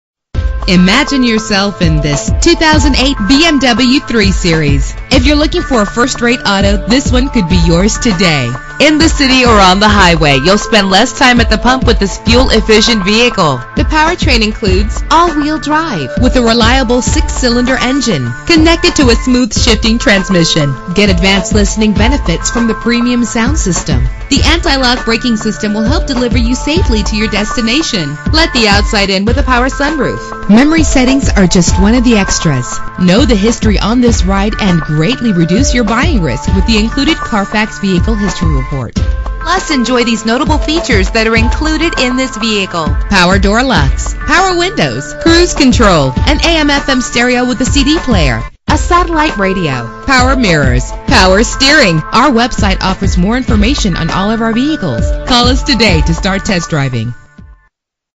2008 BMW 3 Series - Peabody MA
Year: 2008
Make: BMW
Model: 3 Series
Trim: 335xi
Engine: 3.0 liter inline 6 cylinder 24 valve
Transmission: Autostick
Color: Monaco Blue Metallic
Mileage: 22323
Address:
218 Andover Street